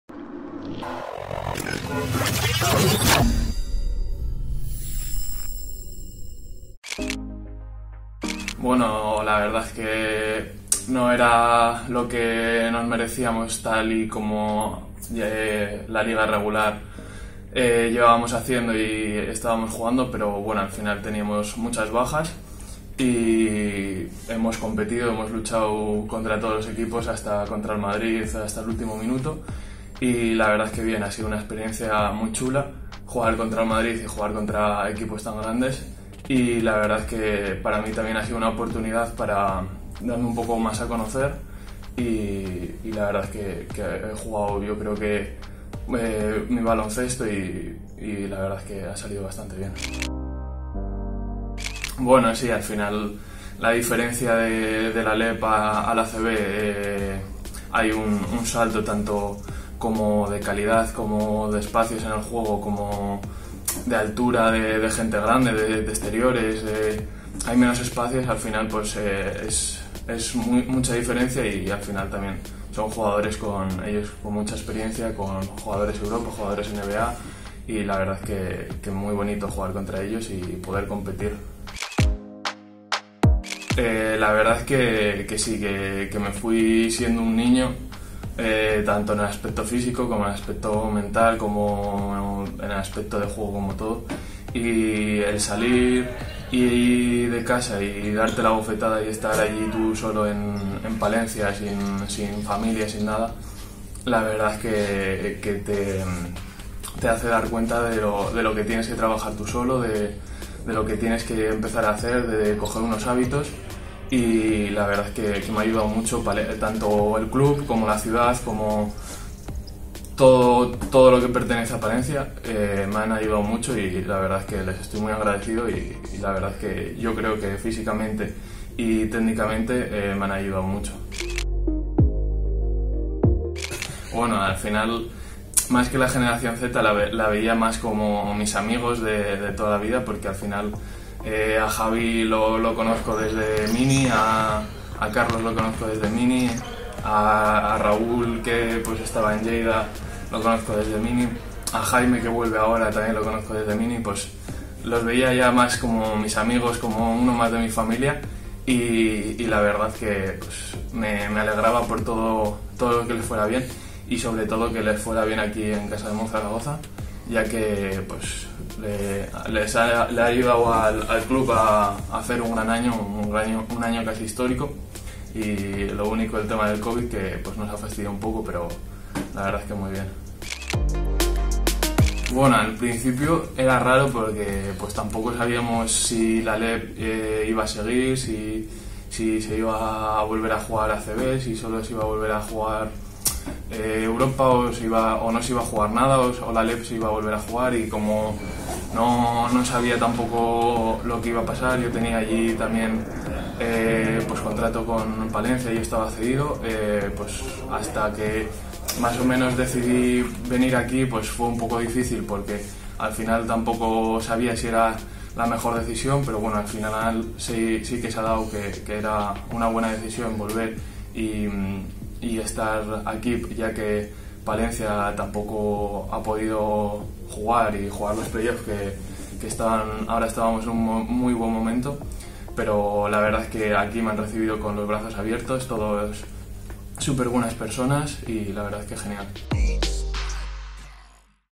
Pradilla: "Volver a casa ha sido la mejor opción"
El joven interior de Casademont Zaragoza ha sido una de las sensaciones de la fase final de la Liga Endesa disputada en Valencia, tras su etapa de cesión en el Chocolates Trapa Palencia.